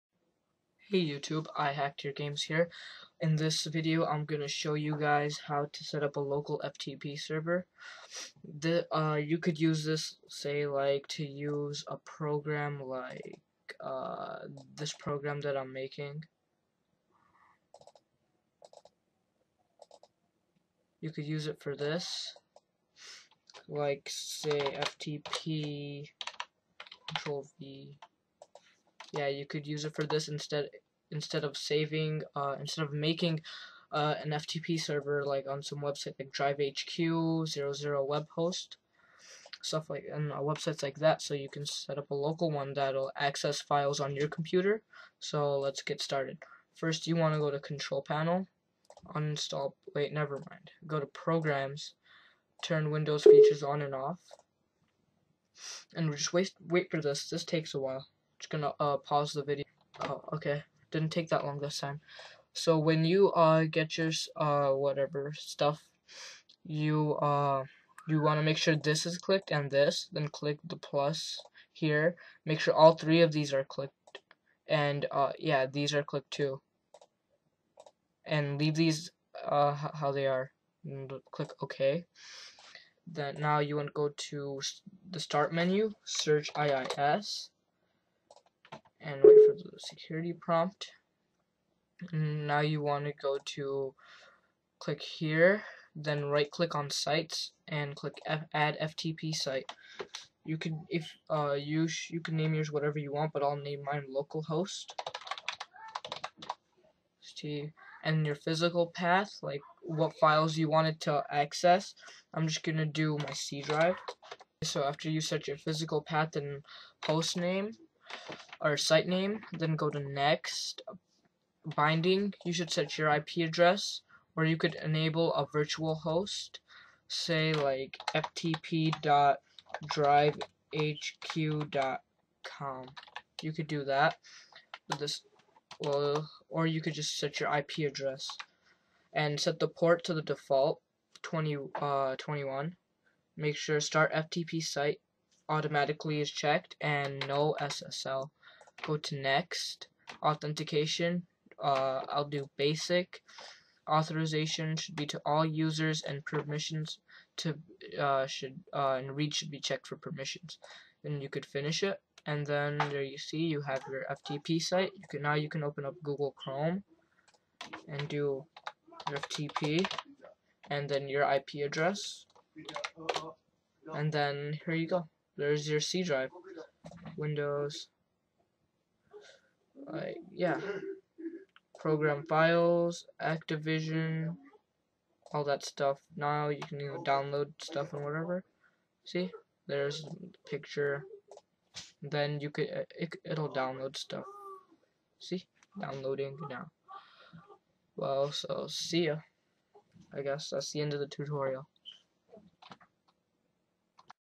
How to Setup a Local FTP Server in Windows 7!
This tutorial will show you how to setup a local ftp server [File transfer Protocol] in windows 7.